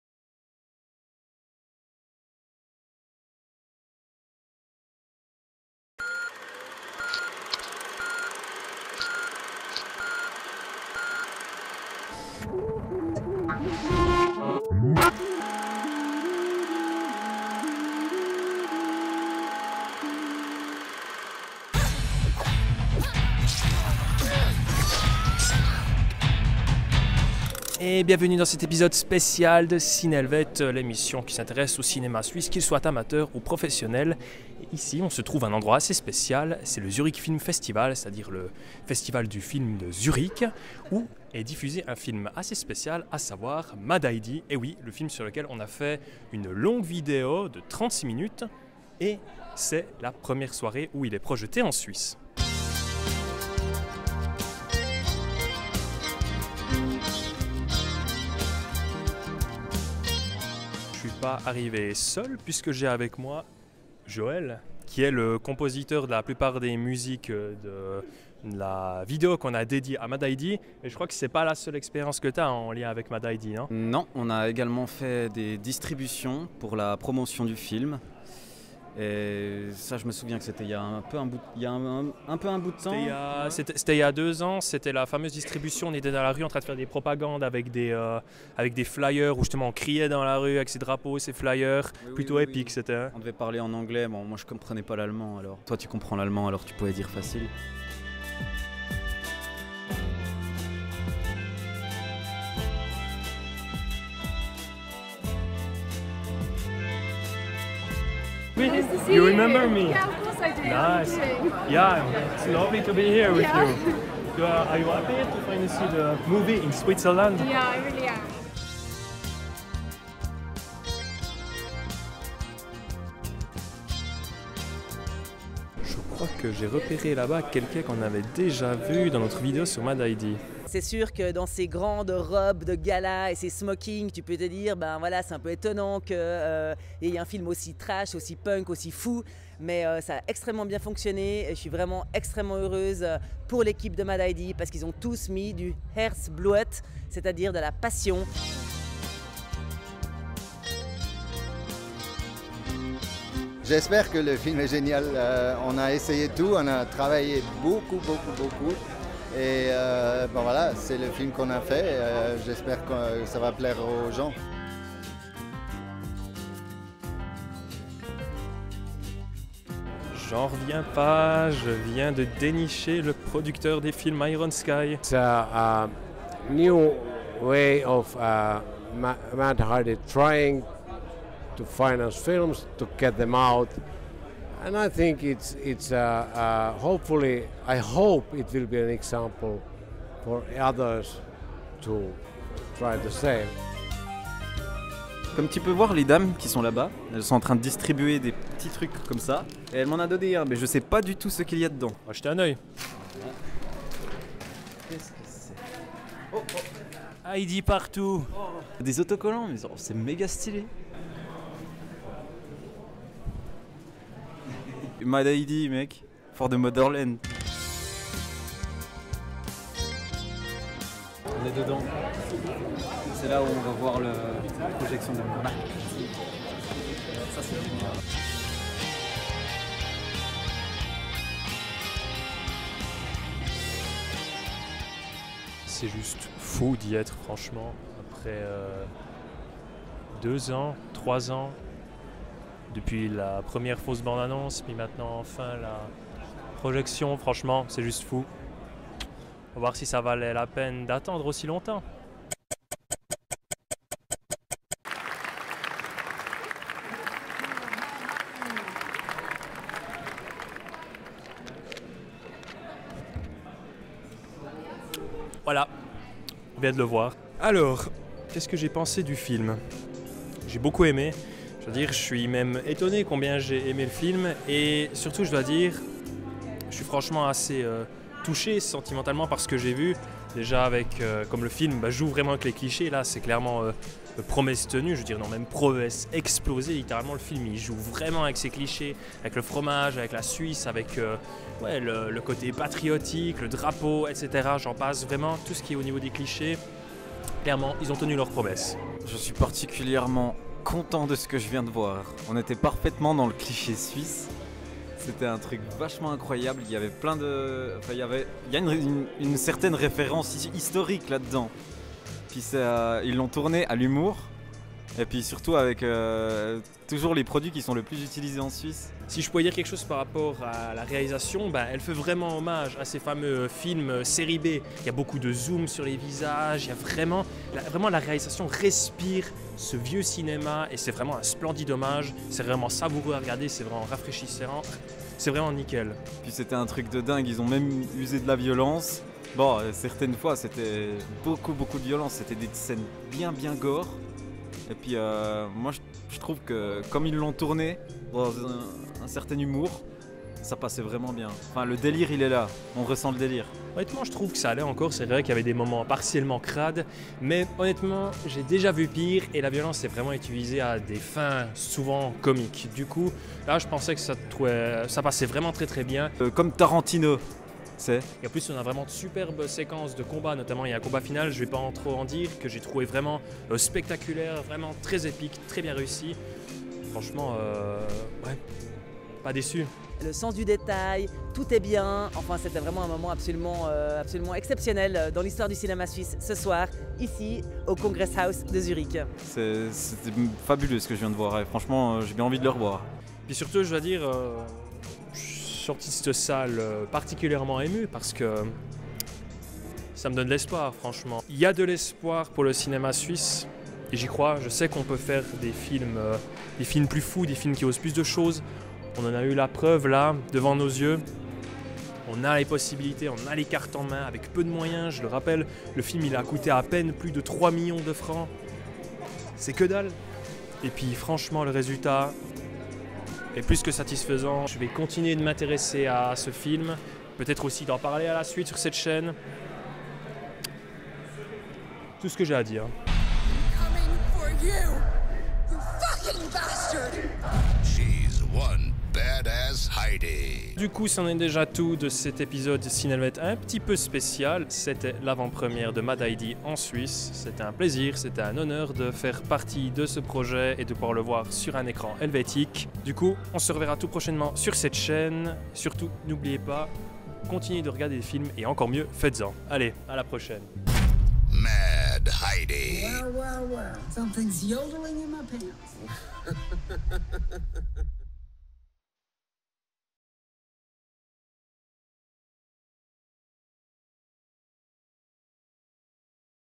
Cinhelvète - MAD HEIDI : L'avant-première suisse
À l'occasion de la sortie de MAD HEIDI, Studio Paroche s'est rendu à l'avant-première suisse du film au Zurich Film Festival 2022. Au programme : du tapis vert, des figurants, des producteurs heureux, des spectateurs trop objectifs et une Heidi flamboyante !

On the occasion of the release of MAD HEIDI, Studio Paroche went to the Swiss premiere of the film at the Zurich Film Festival 2022. On the program: green carpet, extras, happy producers, too objective spectators and a Heidi on fire !